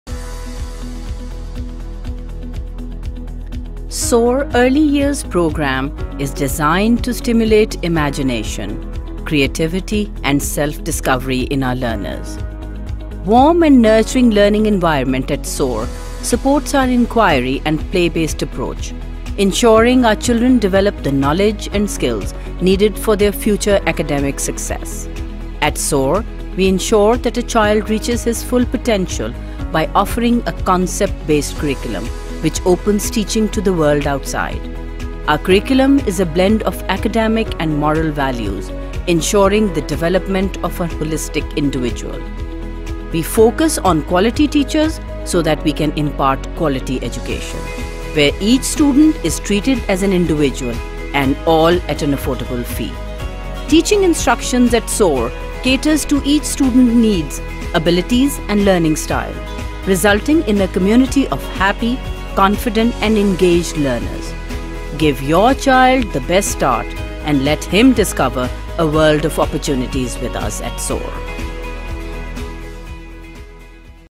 SOAR Early Years Programme!
SOAR Early Years programme is designed to stimulate imagination, creativity and self-discovery in our learners. The warm and nurturing learning environment at SOAR supports our inquiry and play-based approach, ensuring our children develop the knowledge and skills needed for their future academic success.